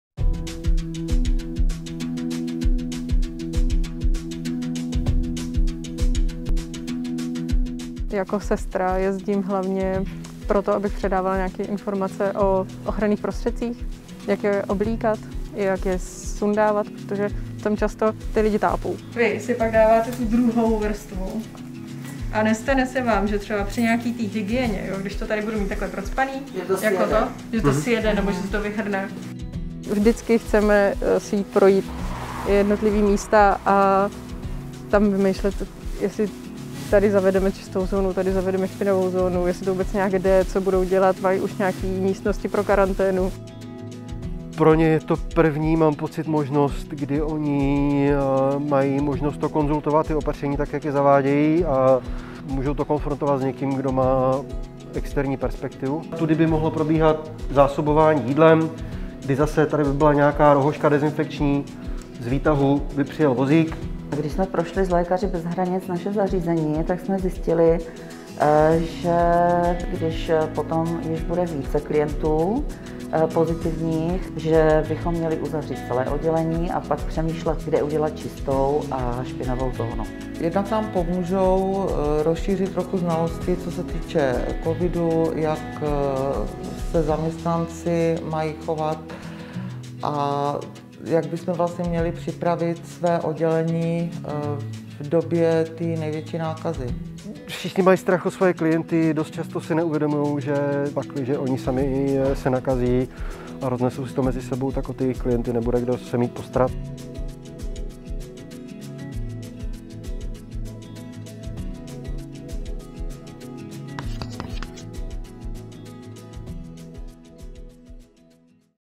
Lékaři bez hranic poprvé pomáhají v ČR
Lékaři bez hranic od počátku listopadu pomáhají v pobytových zařízeních sociálních služeb v ČR. Nabízí jim pomoc s nastavením a zlepšením opatření proti šíření COVID-19: lépe porozumět tomu, jak nákazu kontrolovat; jak zacházet s ochrannými pomůckami, odpadem, prádlem atp.; jak se starat o klienty v izolaci nebo je převážet do nemocnic.

„Když jsme prošli s Lékaři bez hranic naše zařízení, tak jsme zjistili, že když bude více pozitivně testovaných klientů, že bychom měli uzavřít celé oddělení, a pak přemýšlet, kde udělat čistou a špinavou zónu,“ popisuje ředitelka Domova pro seniory v Božicích.

Pro řadu zařízení je naše návštěva první možnost, kdy si mohou s někým ověřit, zda sebe a své klienty chrání dostatečně a jak to případně dělat lépe.